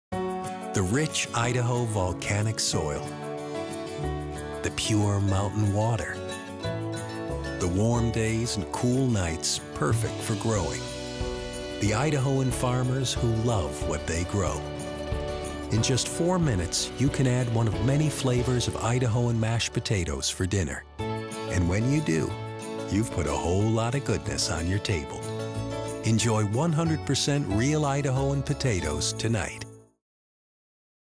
Idahoan TV Ad: Idahoan On Your Table
Idahoan takes you the heart of potato country to show you the 100% goodness that goes into each serving of our mashed potatoes. In just 4 minutes you can enjoy a wide variety of flavors that are a tasty alternative to rice or pasta. Sit down to dinner tonight with all the homemade tastes of Idahoan on your table.